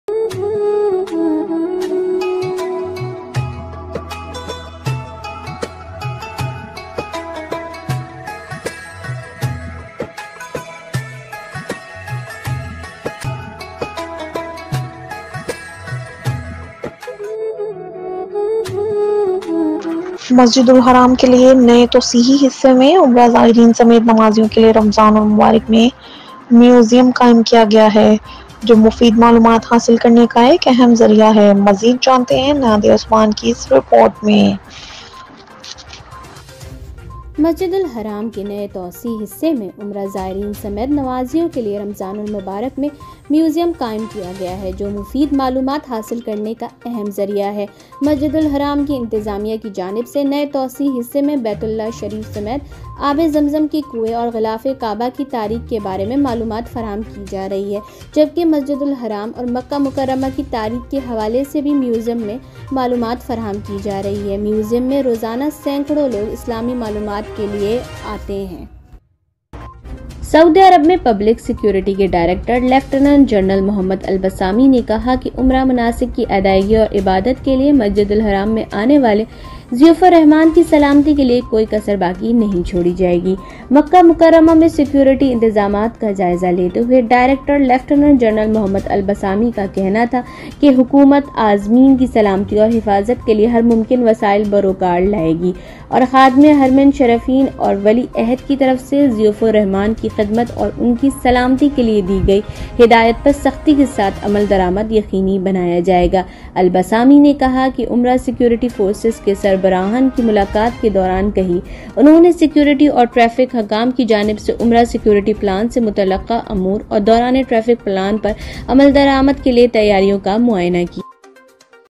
Scheduled shuttle bus service to various places in Makkah, Saudi Arabia
In Saudi Arabia, 17 routes of shuttle bus service have been appointed for transportation from different places of Makkah to the Haram Mosque during Ramadan.

In the new extension of Masjid al-Haram, a museum has been established for pilgrims including Umrah pilgrims in Ramadan, which is an important source of useful information,